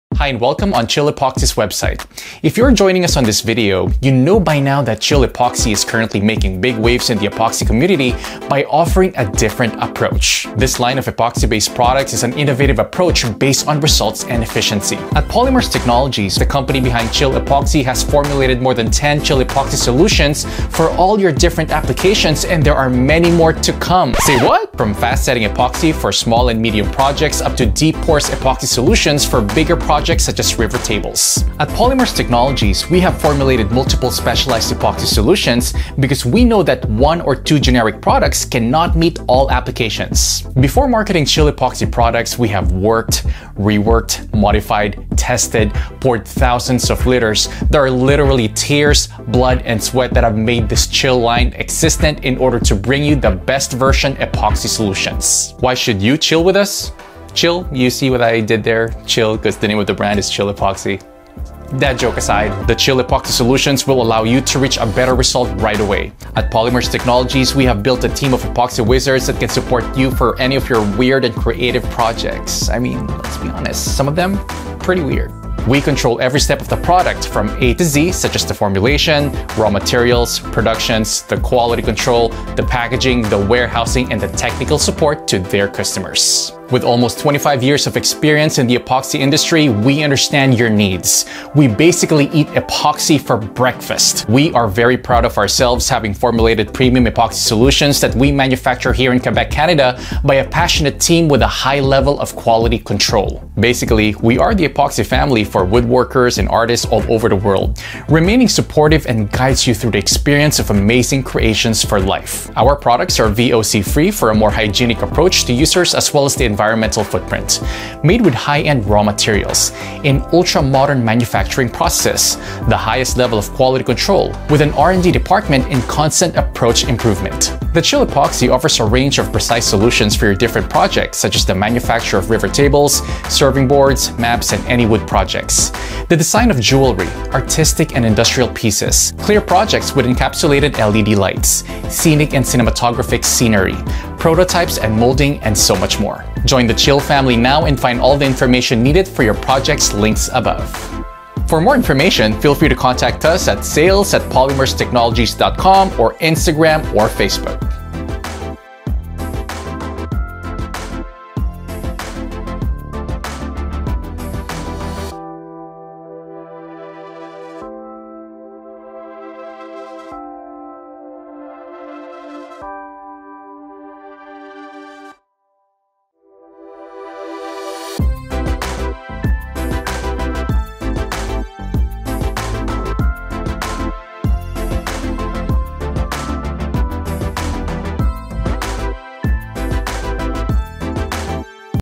CHILL EPOXY™ website is now available - All you need to know about resins
IT’S ALIVE!

You can now visit our new website.

We are very excited to announce the launch of our newly designed website. After months of hard work and dedication we are delighted to officially launch it.

You can find more information on our CHILL EPOXY resins and other accessories.

Tips and Youtube videos - who wants to read anyways right? 😂

Where to buy? And so much more!

TEAM
POLYMERES TECHNOLOGIES